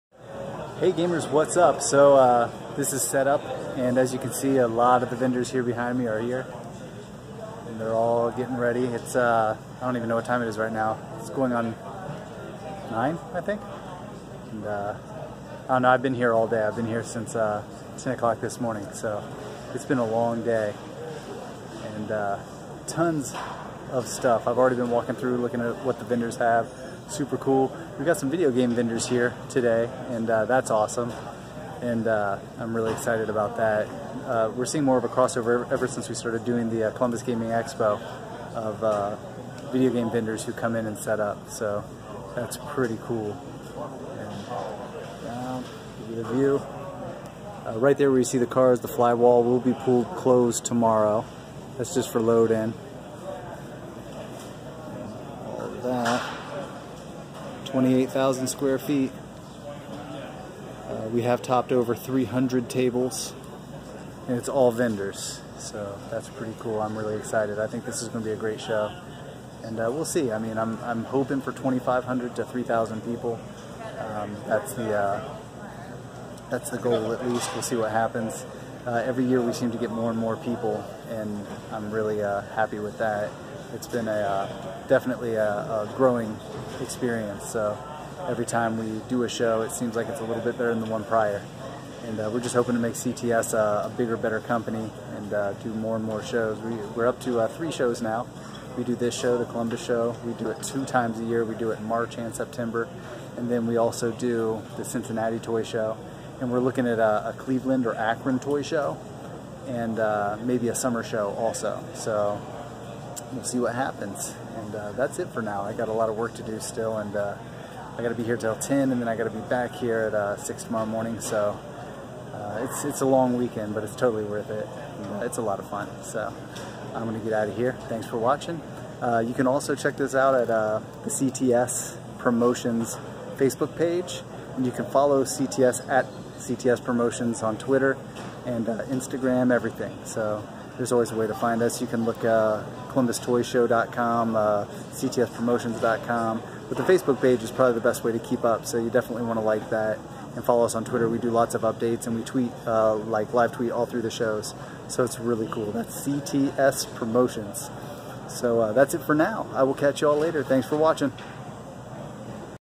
Update from The Columbus Toy Show setup.
The show is getting set up, lots of vendors are in and we're getting geared up for another great turnout.